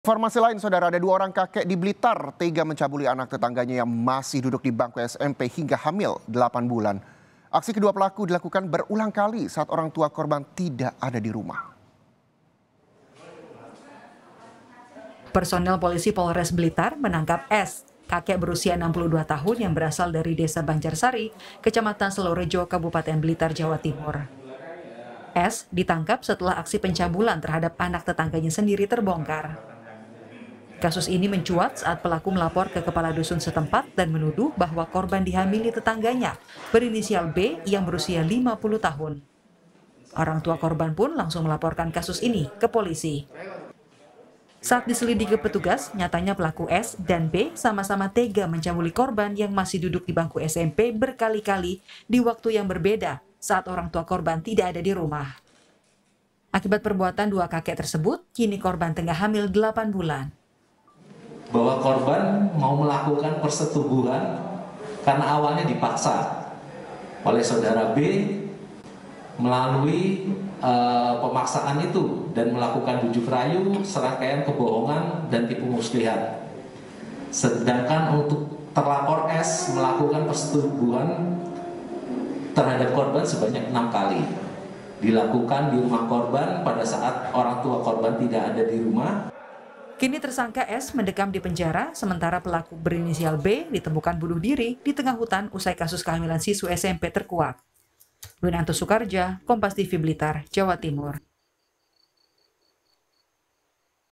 Bejatnya! 2 Kakek di Blitar Perkosa Siswi SMP Hingga Hamil 8 Bulan
BLITAR, KOMPAS.TV - Dua orang kakek di Blitar, tega mencabuli anak tetangganya yang masih duduk di bangku SMP hingga hamil 8 bulan.

Aksi kedua pelaku dilakukan berulang kali saat orangtua korban tidak ada di rumah.

Personel Polisi Polres Blitar menangkap S Kakek berusia 62 tahun yang berasal dari Desa Banjarsari, kecamatan Selorejo, kabupaten Blitar, Jawa Timur.

S ditangkap setelah aksi pencabulan terhadap anak tetangganya sendiri terbongkar.

Baca Juga Hendak Angkut Pasien dari Pedalaman, Helikopter Puskesmas Keliling Jatuh di Mimika, 1 Orang Hilang di

Kasus ini mencuat saat pelaku S melapor ke kepala dusun setempat dan menuduh bahwa korban dihamili tetangganya berinisial B yang berusia 50 tahun.

Orang tua korban langsung melaporkan kasus ini ke polisi.

Saat diselidiki petugas, nyatanya pelaku S dan B sama sama tega mencabuli korban yang masih duduk di bangku SMP berkali kali di waktu yang berbeda saat orang tua korban tidak ada di rumah.

Akibat perbuatan ke 2 kakek tersebut kini korban tengah hamil 8 bulan.

Baca Juga Akibat Sengketa Lahan, Siswa SDN 356 di Luwu Sulawesi Selatan Terpaksa Belajar di Rumah Guru di